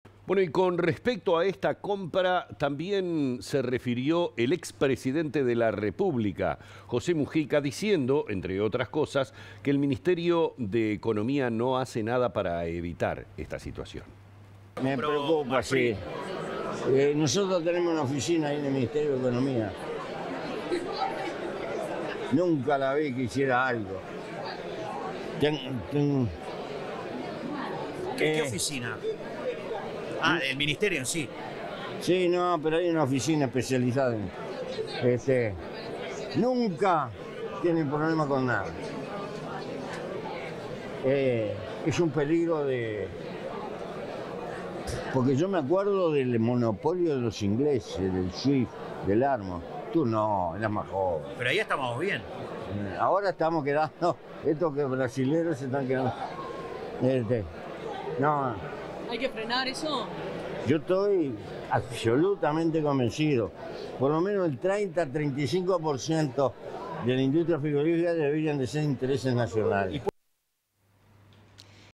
José Mujica: “Hay una oficina en economía que nunca hace nada”
El expresidente José Mujica se refirió a la compra que realizó la empresa brasileña Minerva Foods y dijo que el Ministerio de Economía no hace nada para evitar la situación.